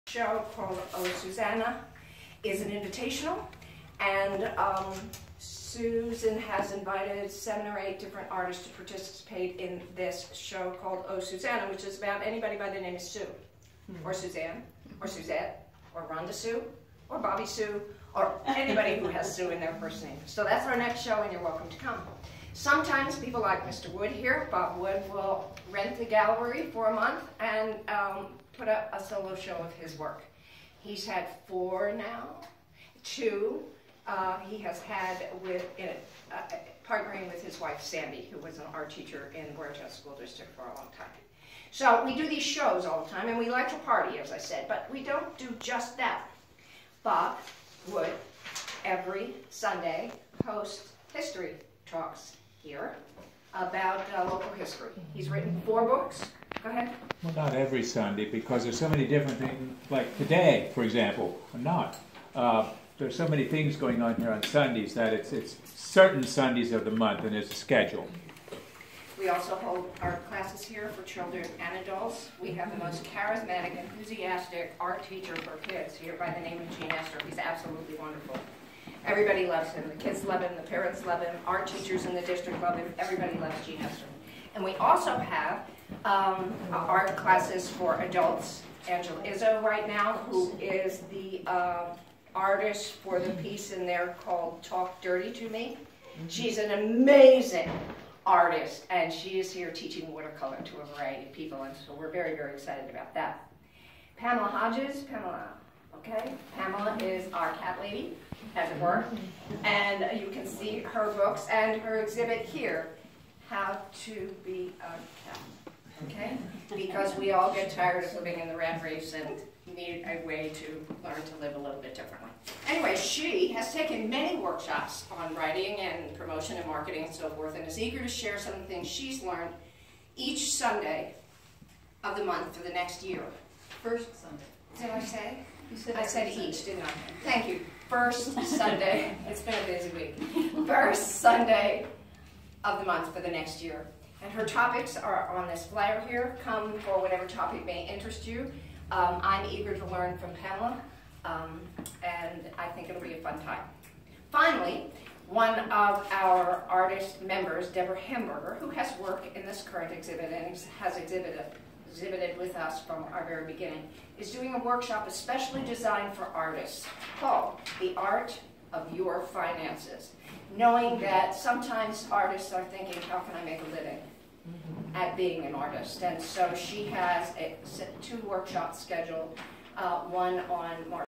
Jane Stahl Talks About Studio B
Sunday, February 19, 2017. Jennifer Hetrick hosts a poetry reading at Studio B in Boyertown, PA. The participants present a mix of their joy of life and an occasional cathartic sorrow. Laughter and tears reach the rafters and bounce off the floor, in that order. Just what the word-doctor ordered. Beauty obtains. Healing happens. We are a-MUSE-d. Here, Jane Stahl talks about Studio B and its role in the Boyertown community.